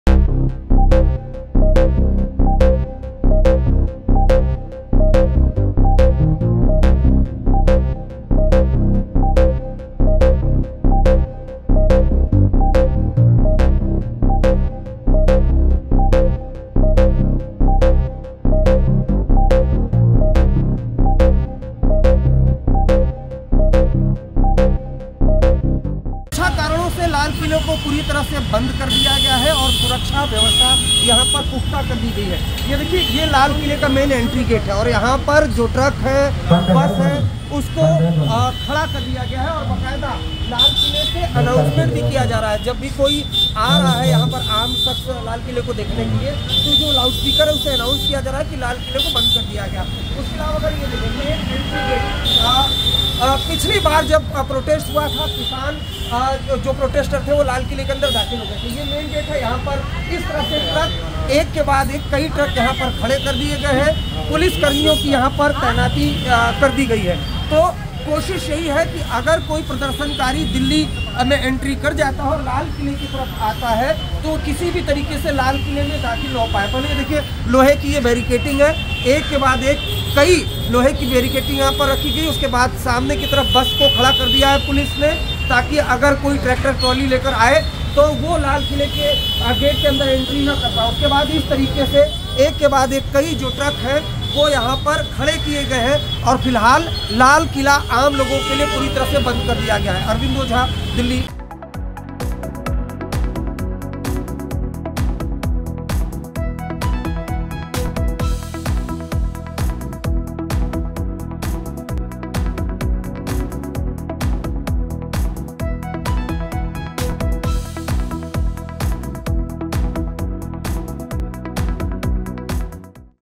Farmer Protest को देखते हुए सरकार ने Lal Quila को लेकर बड़ा फ़ैसला ले लिया। Haryana Tak
Farmer Protest को देखते हुए सरकार ने Lal Quila को लेकर बड़ा फ़ैसला ले लिया।